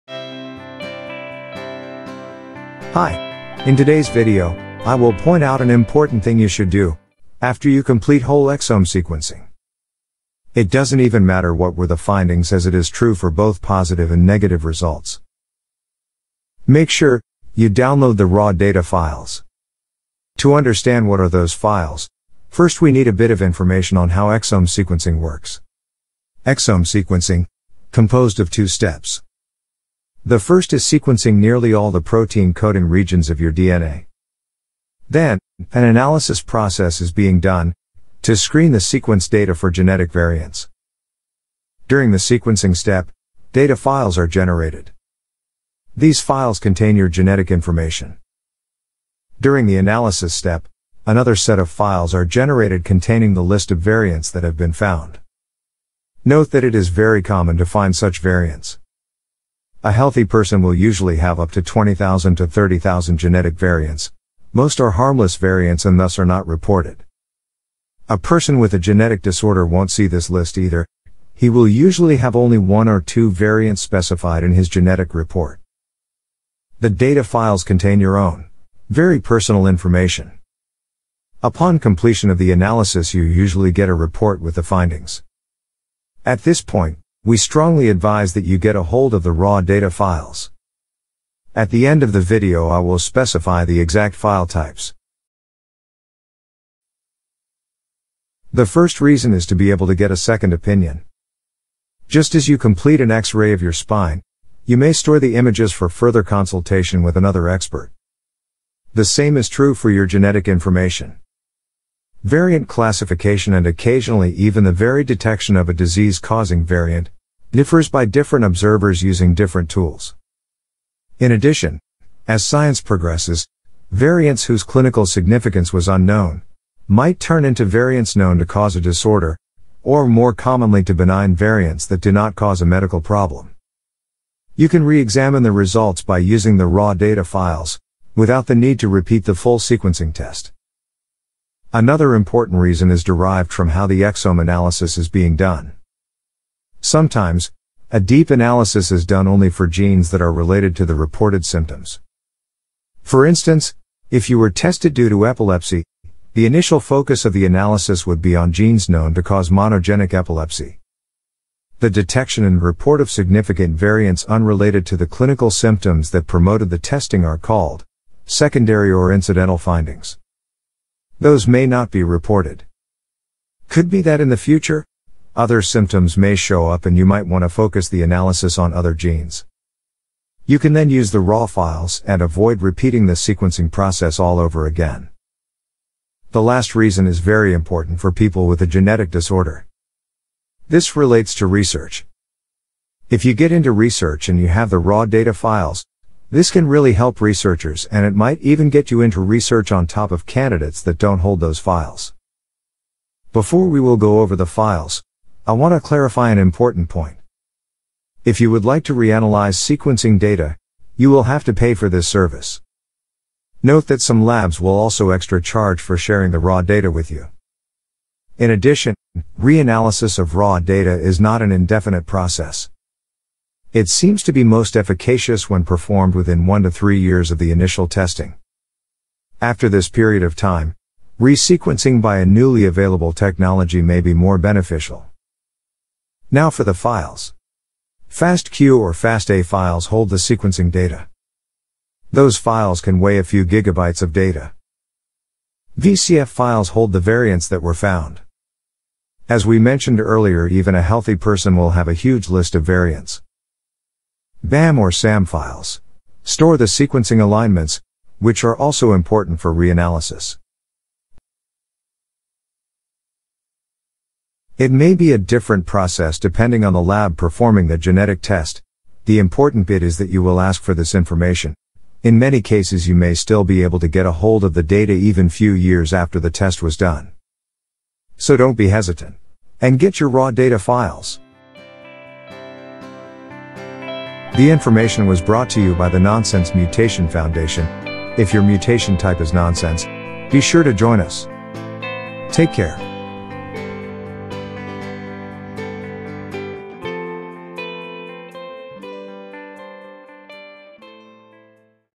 How Does Whole Exome Sequencing Work 🧬 Get The Raw Data Files ⚕️
What is whole exome sequencing? What is importance of whole exome sequencing?
▶️ If you uncertain what is your mutation type, follow the instructions on our
Hello Guys! Welcome and in this video we will discuss about whole exome sequencing test, and the importance of picking up the data files.
If you or your kid went through a whole exome sequencing test, were you aware of the importance of picking up the data files? Even if you haven't done it right away, in most cases you can still get your data files.
Whole-exome sequencing is a popular next-generation sequencing (NGS) approach for sequencing the genome's protein-coding regions. The human exome makes up less than 2% of the genome but contains 85% of known disease-related variants1, making it a more cost-effective method than whole-genome sequencing.
Exome enrichment and exome sequencing can be used to quickly detect coding variants in a variety of applications, including population genetics, genetic illness, and cancer research.
▶️ Advantages of Exome Sequencing:
• Variants are detected across a wide range of applications.
• Ensures that all coding regions are covered.
• Exome sequencing is a less expensive alternative to whole-genome sequencing (4–5 Gb per exome vs. 90 Gb per full human genome).
• When compared to whole-genome techniques, produces a smaller, more manageable data collection for faster, easier data analysis.
▶️ Efficient Analysis of Coding Regions
When whole-genome sequencing is neither feasible or essential, exome sequencing provides a cost-effective alternative. Only sequencing the coding portions of the genome allows researchers to concentrate their efforts on the genes that are most likely to influence phenotype, while also providing an affordable mix of turnaround time and cost.
Exome sequencing detects variations in coding exons and can be expanded to include untranslated regions (UTRs) and microRNA for a more complete picture of gene regulation. DNA libraries can be made in as little as one day, and each exome only requires 4–5 GB of sequencing space.